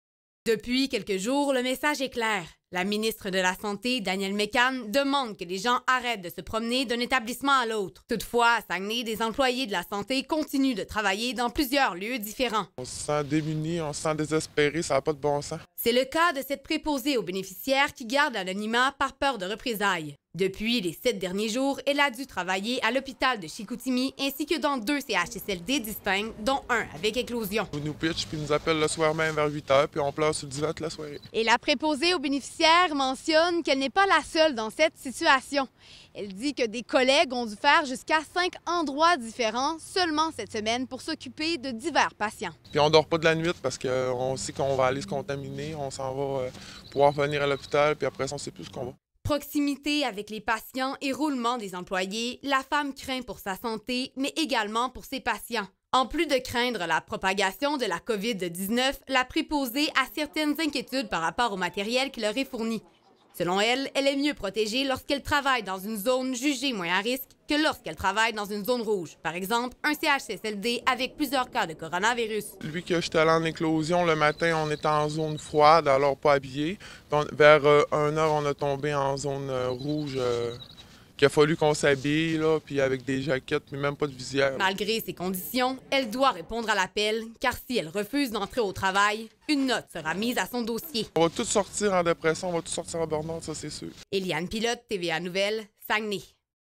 Une préposée aux bénéficiaires mutée d’un établissement à l’autre
Depuis plusieurs jours, le message est clair : la ministre de la Santé Danielle McCann demande que les gens arrêter de se promener d’un établissement à l’autre.

Toutefois, à Saguenay, des travailleurs de la santé continuent de travailler dans plusieurs lieux différents.

C’est le cas d’une préposée aux bénéficiaires qui préfère garder l’anonymat par peur de représailles.

Elle souhaite dénoncer la situation dans laquelle elle se trouve. Depuis les 7 derniers jours, elle a dû se rendre dans trois endroits différents pour s’occuper de divers patients : à l’hôpital de Chicoutimi ainsi que dans 2 CHSLD distincts, dont un avec éclosion.

«On est envoyé d’un bord puis de l’autre, on est appelé à 8 heures le soir pour savoir où nous nous allons le lendemain matin, raconte-t-elle. On braille et on a peur d’être infectée, mais aussi de propager le virus. Moi, j’ai fait 3 endroits en 1 semaine, mais j’ai des collègues qui en ont fait 5 en seulement 7 jours, ça aucun bon sens.»

En plus de craindre de propager la COVID-19, la préposée a certaines inquiétudes par rapport au matériel qui leur est fourni. Selon ces dires, elle est mieux protégée lorsqu’elle travaille dans une zone jugée moins à risque que lorsqu’elle travaille dans une zone rouge.

«À l’hôpital, un endroit considéré comme une zone tiède (avec peu de risque), j’avais accès à une visière! Lorsque je suis allé travailler dans le CHSLD en éclosion, qui est considéré comme une zone rouge, j’étais beaucoup moins protégé», déplore-t-elle.

Malgré ces conditions, elle doit répondre à l’appel, car si elle refuse d’entrer au travail, son employeur a été clair : une note sera mise à son dossier.

«Si ça continue de même, on va tous finir en burn-out, c’est sûr», affirme-t-elle.